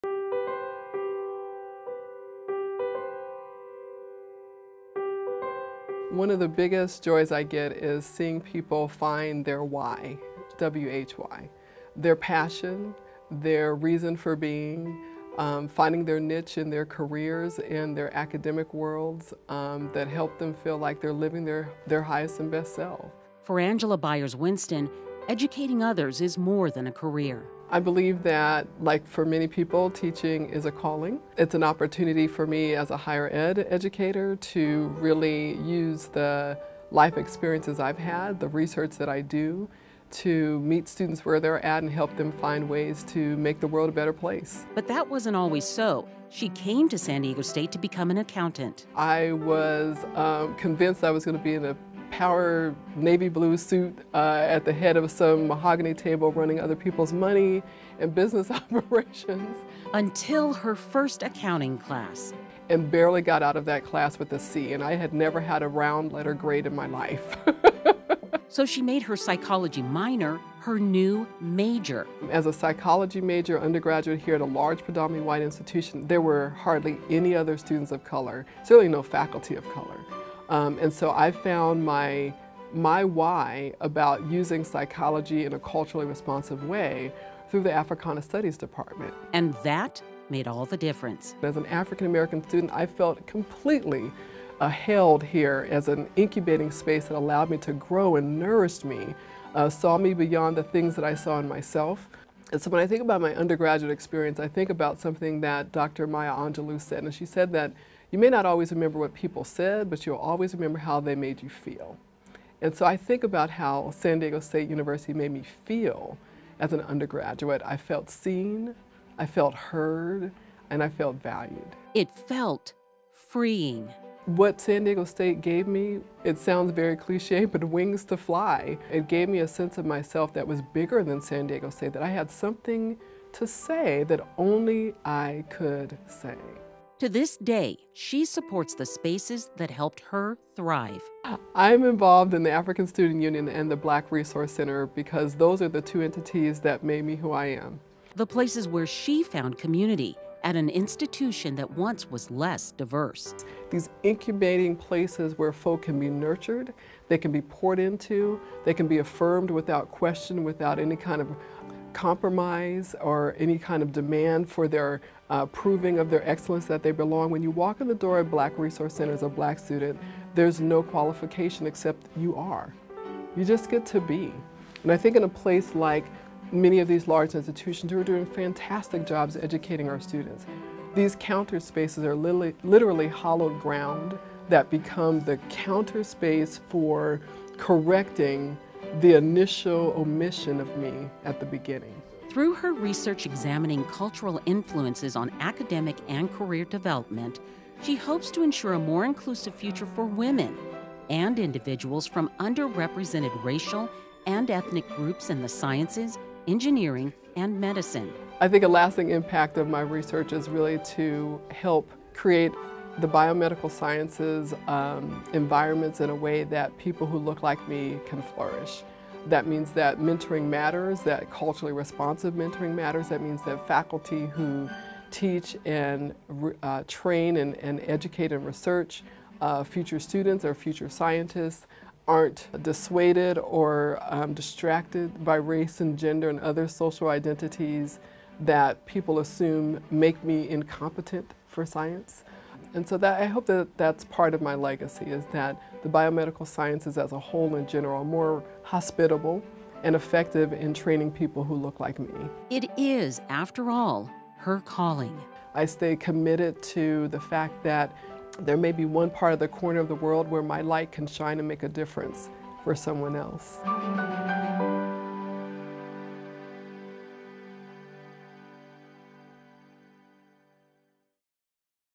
Angela Byars-Winston, Ph.D. | SDSU Alumni Awards of Distinction
Angela Byars-Winston, Ph.D. (’91, psychology, Spanish; ’92 counselor education), professor of internal medicine, is director of research and evaluation in the University of Wisconsin-Madison Center for Women’s Health Research. An elected fellow of the American Psychological Association and a former member of the National Academy of Sciences’ Board of Higher Education and Workforce, her work to diversify science fields led in 2011 to her designation as a Champion of Change by the White House through President Obama's Winning the Future initiative.